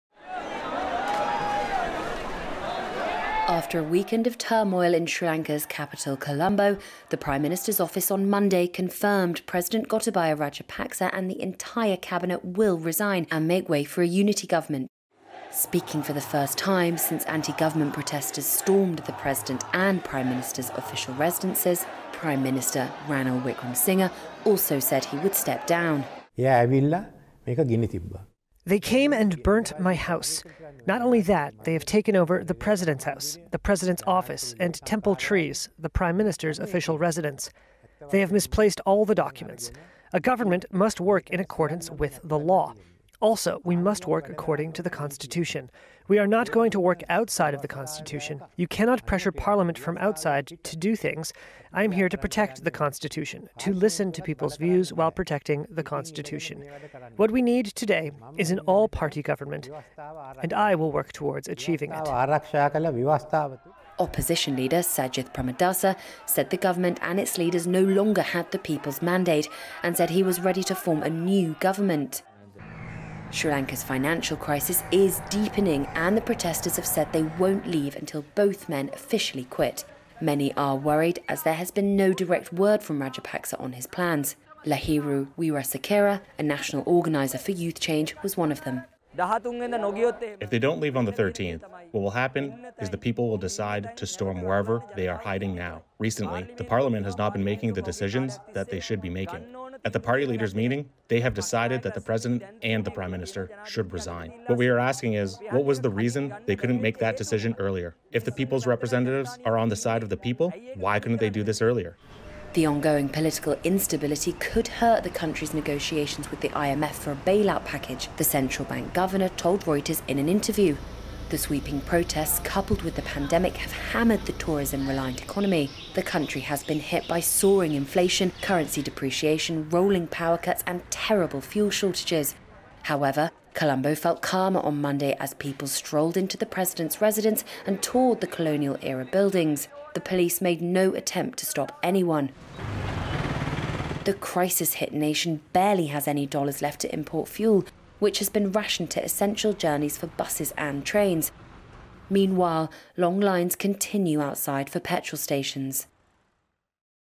Sri Lanka President Rajapaksa confirms resignation
Sri Lanka's President Gotabaya Rajapaksa and the entire cabinet will resign to make way for a unity government, after tens of thousands of protesters stormed the official residences of both men.